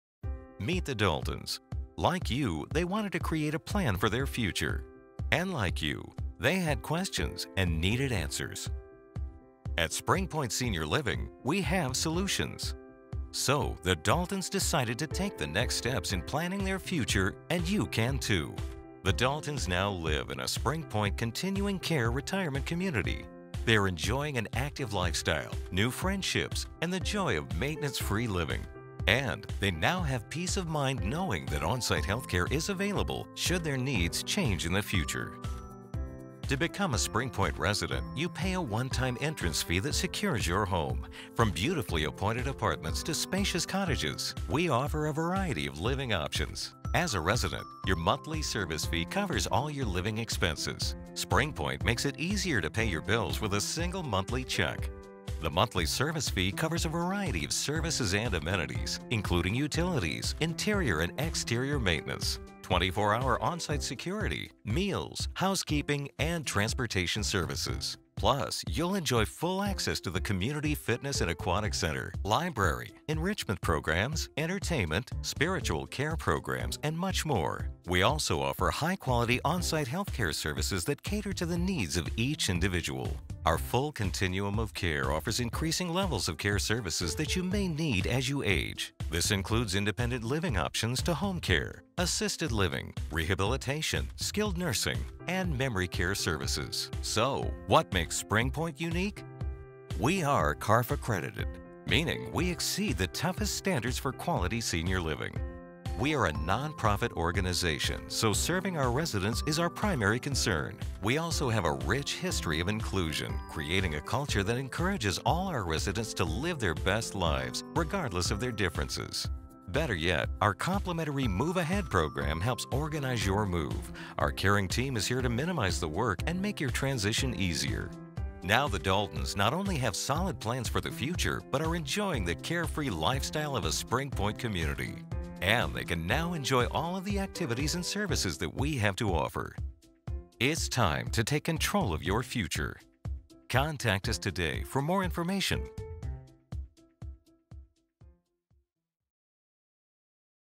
Meet The Daltons
Like you, they wanted to create a plan for their future. And like you, they had questions & needed answers. Listen to what The Daltons learned about our Springpoint Senior Living communities.